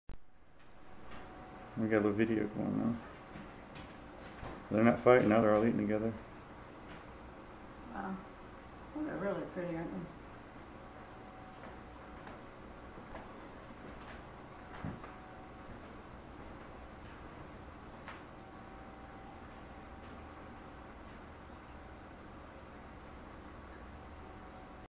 Indigo Bunting family
A family of Indigo Buntings visits a feeding platform near the St. Johns River.